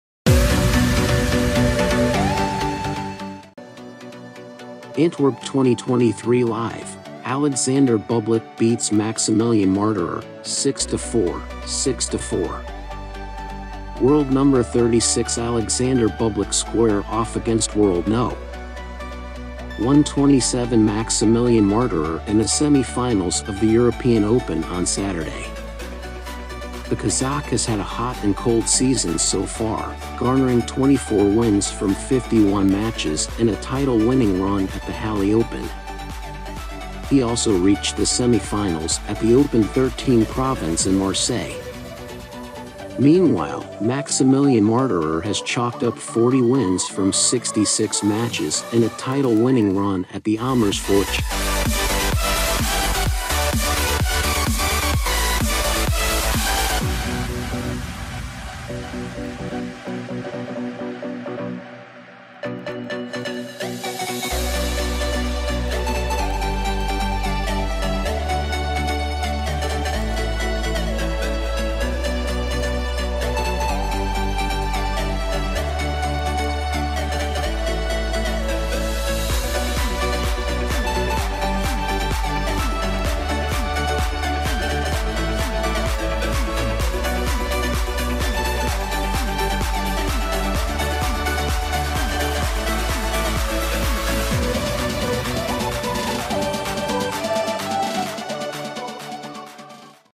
Alexander Bublik vs Maximilian Marterer HIGHLIGHTS EUROPEAN OPEN 2023 🔴 | TENNIS LIVE 🔴
Antwerp 2023 live : Alexander Bublik beats Maximilian Marterer, 6-4, 6-4.

World No. 36 Alexander Bublik square off against World No. 127 Maximilian Marterer in the semifinals of the European Open on Saturday.

The Kazakh has had a hot and cold season so far, garnering 24 wins from 51 matches and a title-winning run at the Halle Open. He also reached the semifinals at the Open 13 Provence in Marseille.

Meanwhile, Maximilian Marterer has chalked up 40 wins from 66 matches and a title-winning run at the Amersfoort Challenger. He also reached the quarterfinals at the Maharashtra Open and the third round at the Wimbledon Championships.